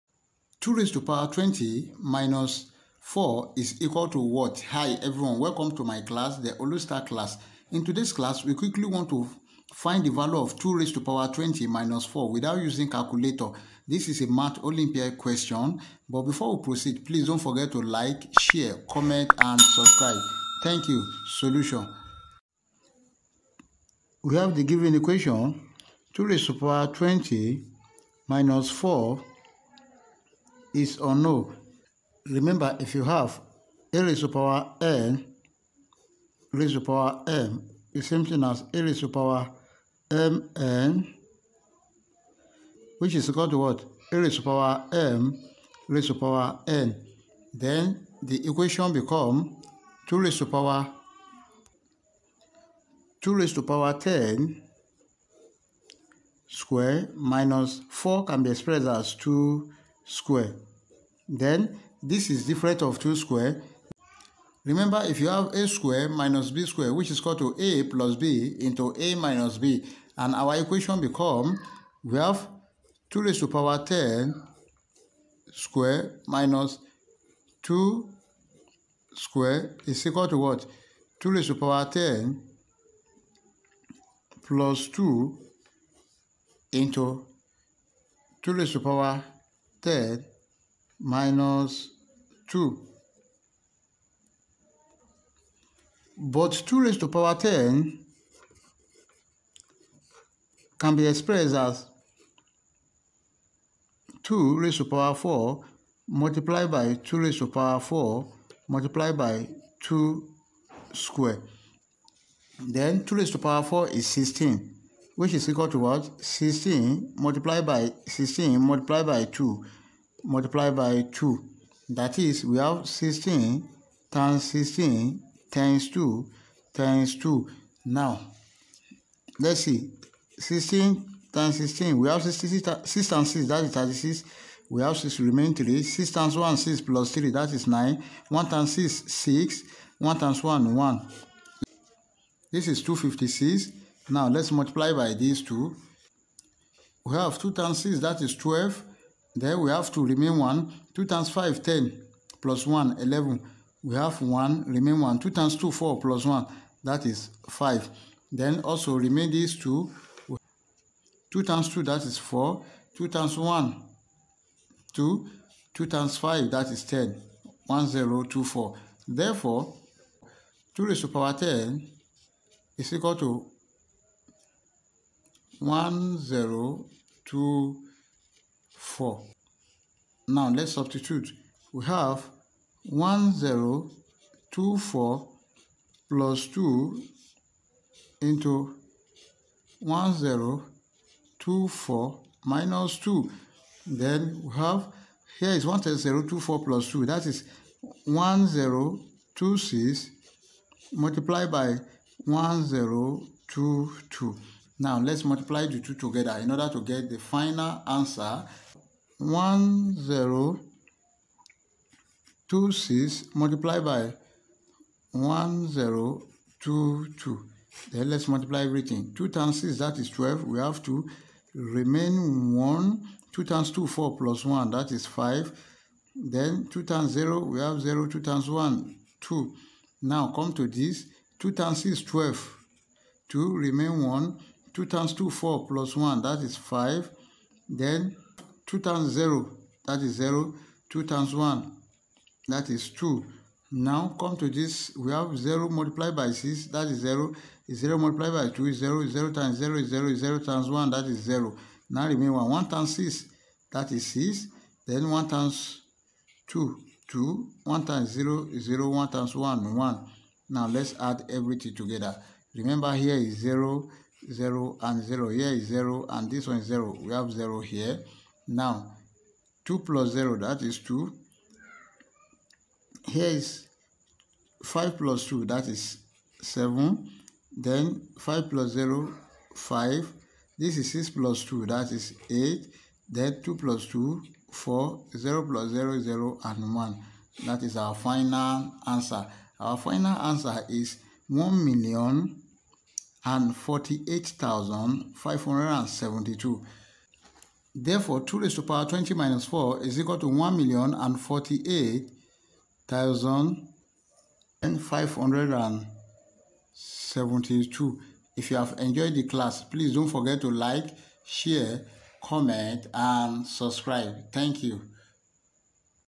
Math Olympiad Challenge 2^20-4 | Best Trick!!!
In this video, you will learn how to calculate faster than calculation in solving this given equation 2^20-4. It can be solve in 1 minute.  It is a math olympiad challenge